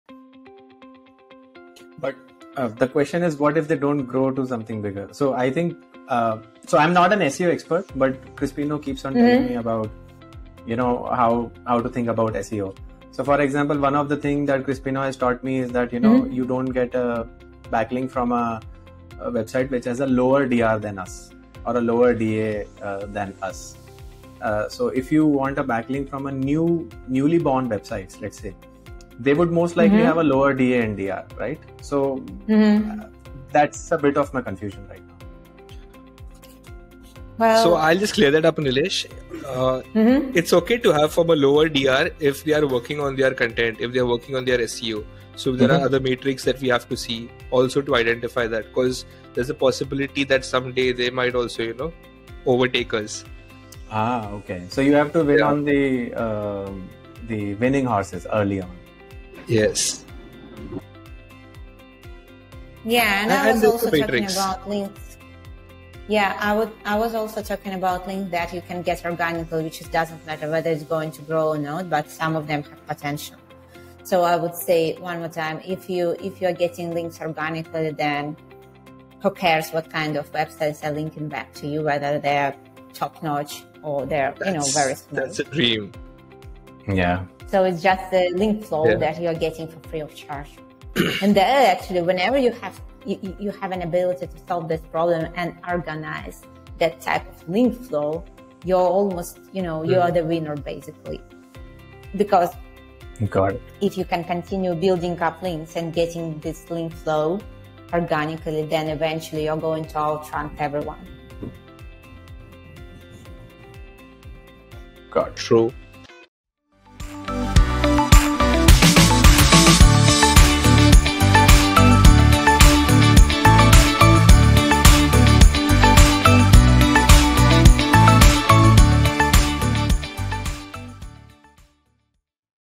SEO Myths Debunked: The Value of New Website Backlinks
SEO Myths Debunked: How New Backlinks Boost Your Site's Ranking! In this video, we dive deep into the world of SEO and unravel common myths about the value of new website backlinks. Discover how fresh backlinks can significantly improve your site's ranking and visibility. We'll explore the importance of backlinks, how they influence search engine algorithms, and why new backlinks are crucial for maintaining a competitive edge.

Whether you're a seasoned SEO expert or a beginner, this video will provide valuable insights and practical tips to enhance your SEO strategy. Don't miss out on this essential guide to understanding and leveraging the power of new backlinks for your website's success. Watch now and take your SEO game to the next level!

Make sure to follow me on these platforms to stay updated with my latest videos and other content.